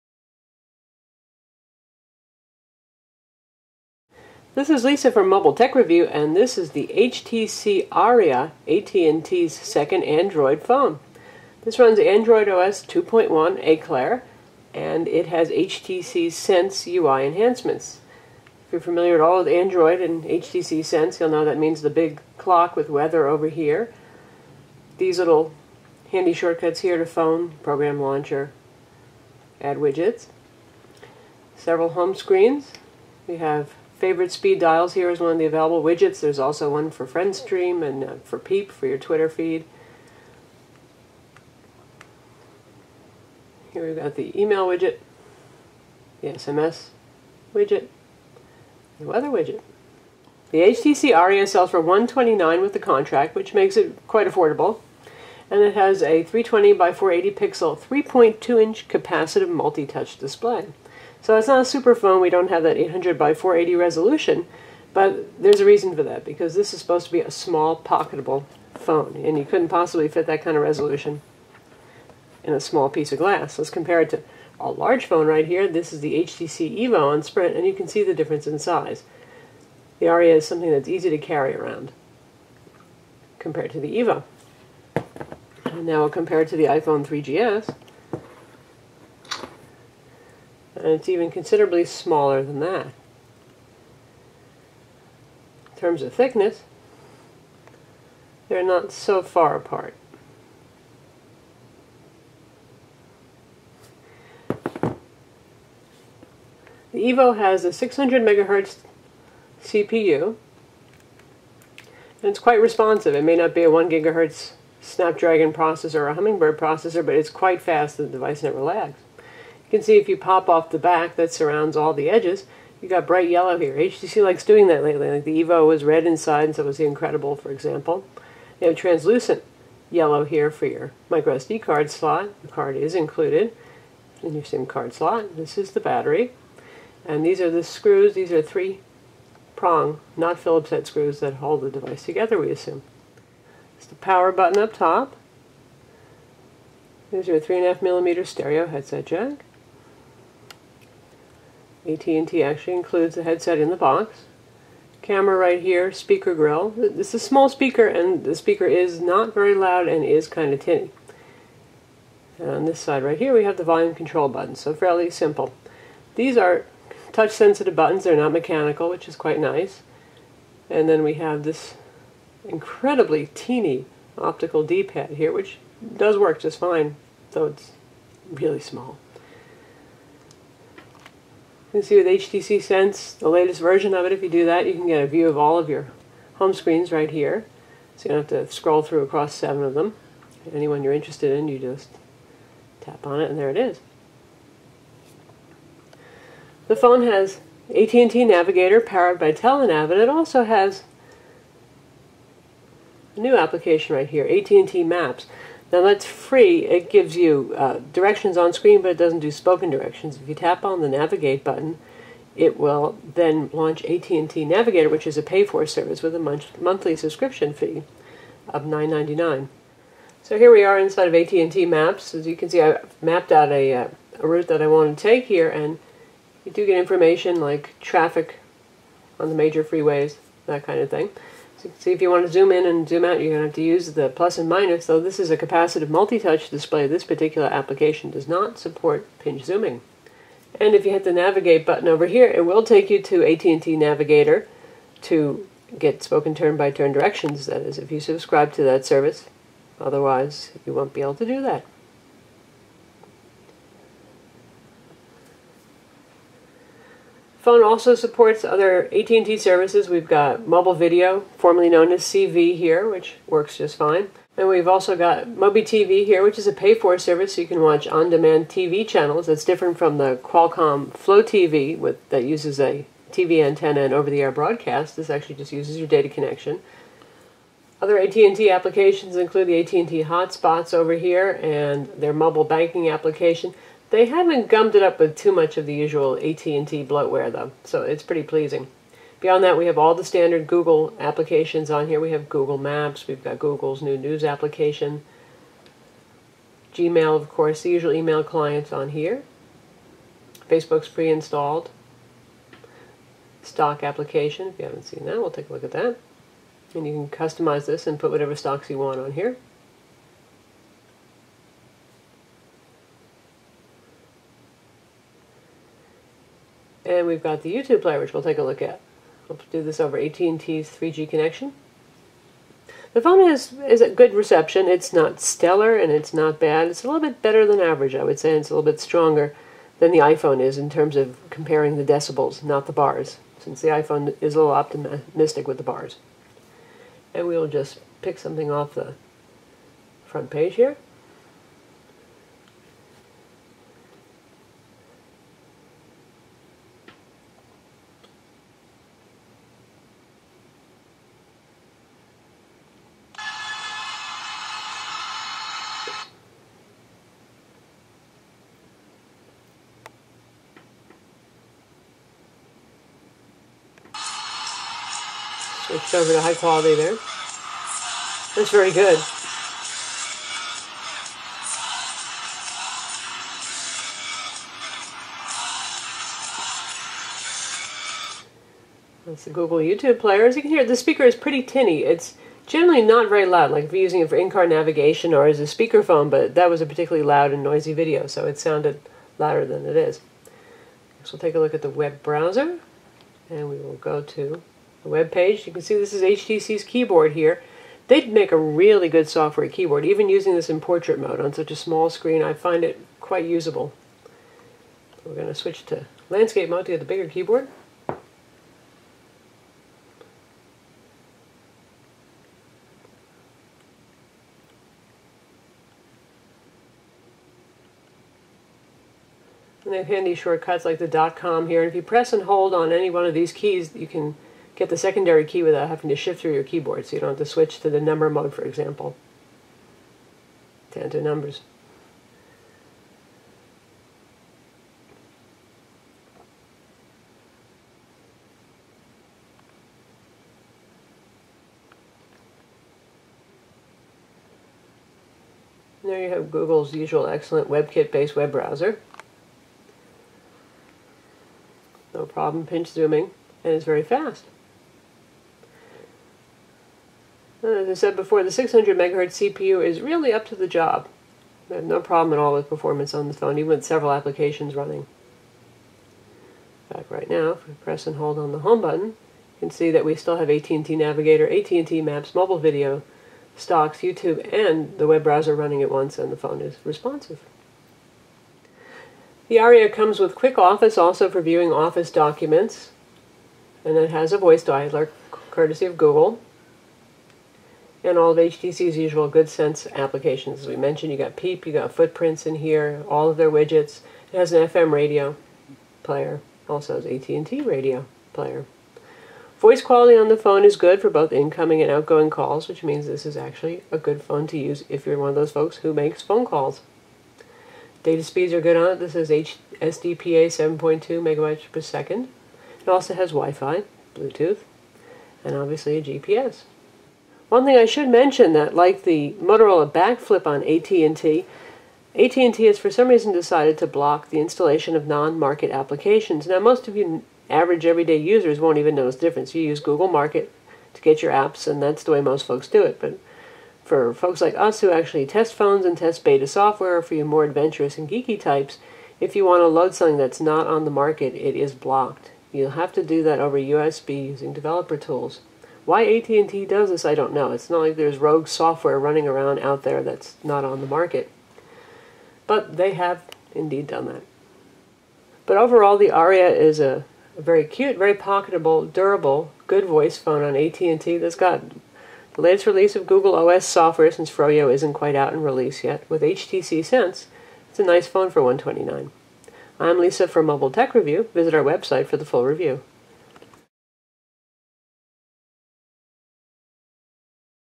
HTC Aria on AT&T Video Review
A video review of the HTC Aria Android smartphone on AT&T. The HTC Aria sells for $129 with a contract, and it runs the Android 2.1 Eclair with HTC Sense. It's pocketable and it has a 3.2" 320 x 480 capacitive multi-touch display. Other features include WiFi, Bluetooth, GPS that works with Google Maps, AT&T Navigator and AT&T Maps, and a 5 megapixel camera with auto focus.